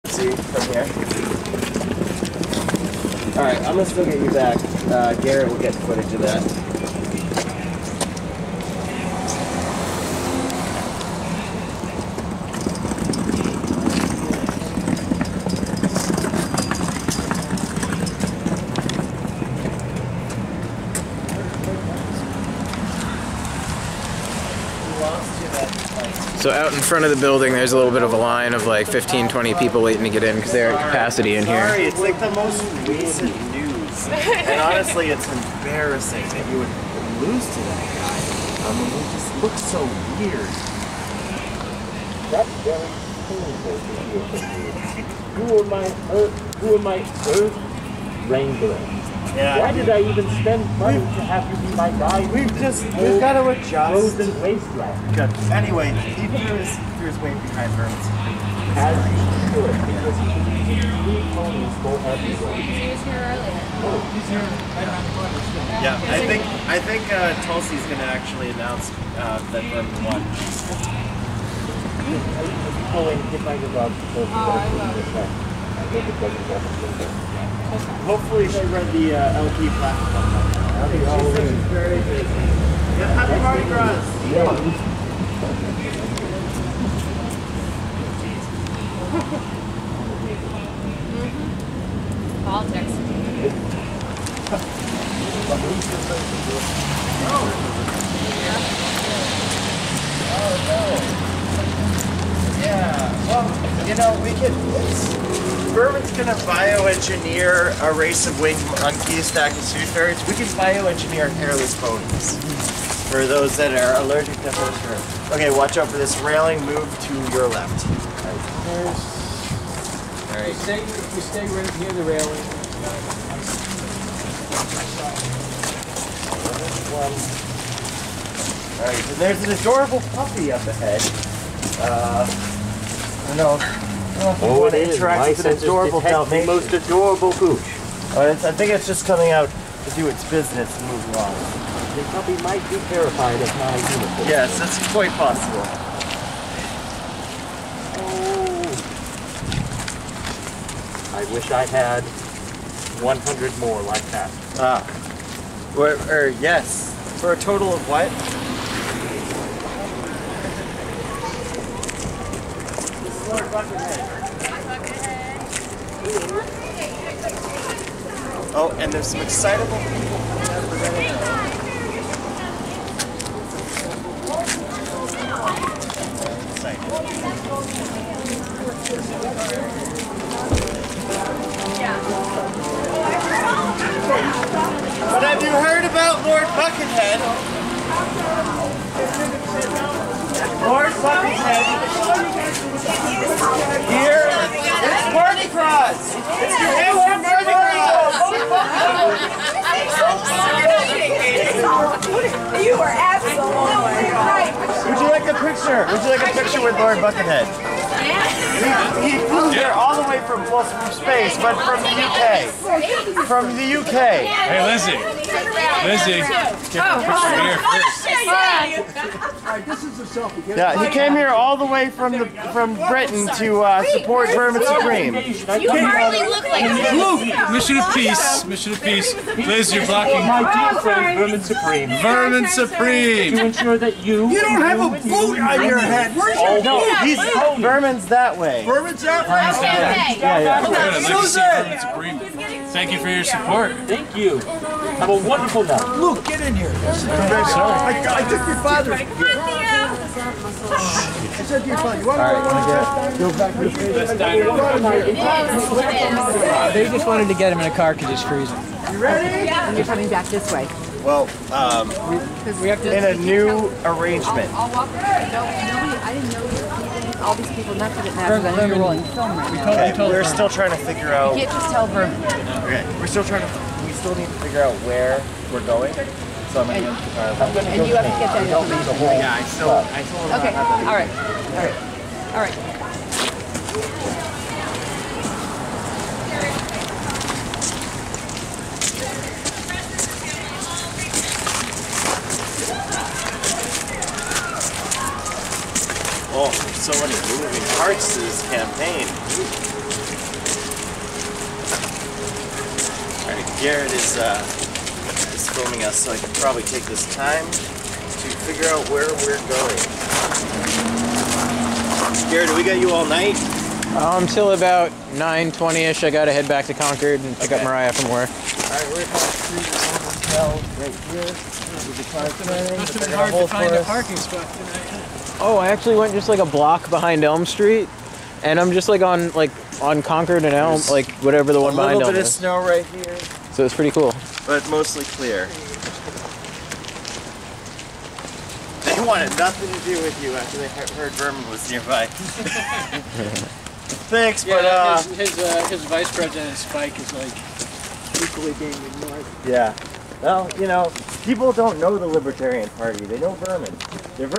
Tulsi's FITN Party w/Vermin Supreme & Lord Buckethead
Penuche's Music Hall, Manchester, NH
After working the crowd inside, Libertarian Presidential candidate Vermin Supreme meets and mingles with fans outside of the packed party for Tulsi Gabbard's democratic presidential candidacy. Lord Buckethead accompanies the campaign to promote Libertarian activist extraordinaire Vermin Supreme.